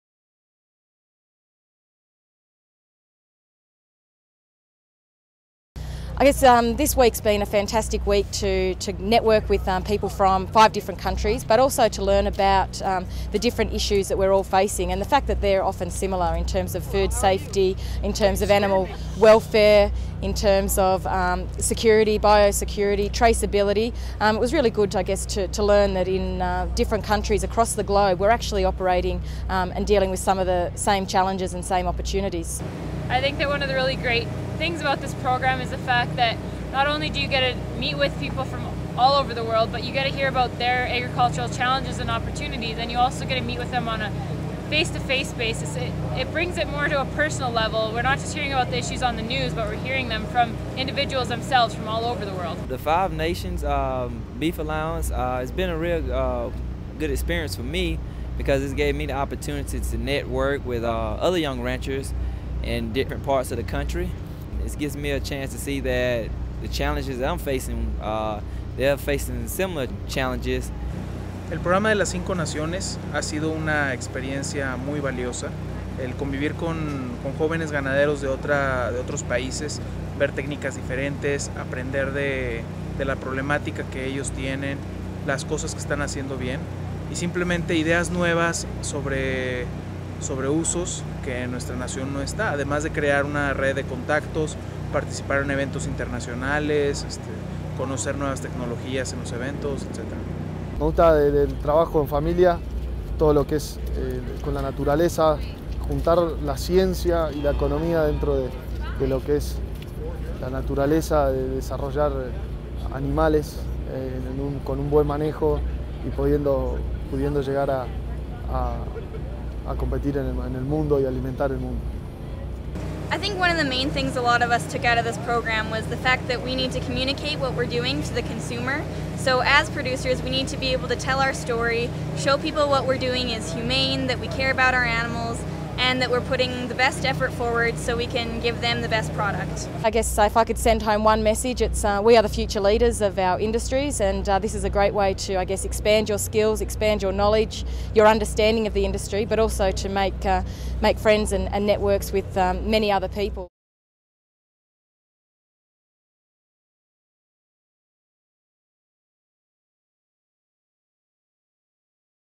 Five Nations Beef Alliance - English version
Young cattlemen from five countries gathered in Denver for the Five Nations Beef Alliance young ranchers program.